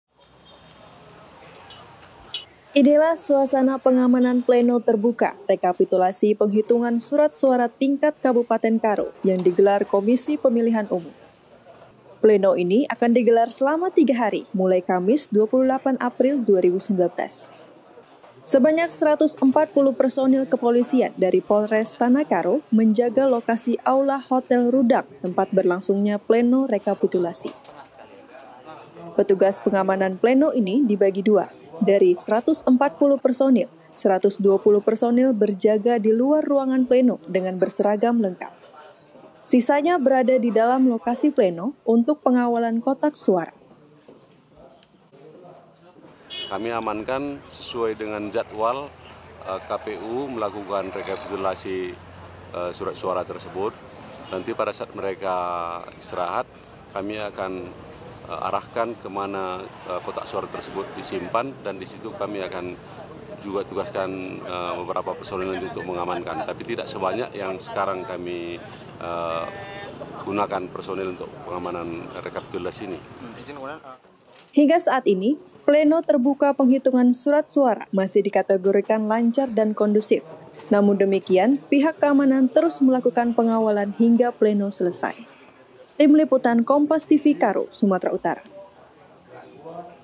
PENGAMANAN REKAPITULASI
PLENO TERBUKA REKAPITULASI PENGHITUNGAN SUARA TINGKAT KABUPATEN KOTA DIJAGA KETAT. POLRES TANAH KARO KERAHKAN 140 PERSONIL 20 PERSONIL MASUK DALAM PENGAMANAN TERTUTUP.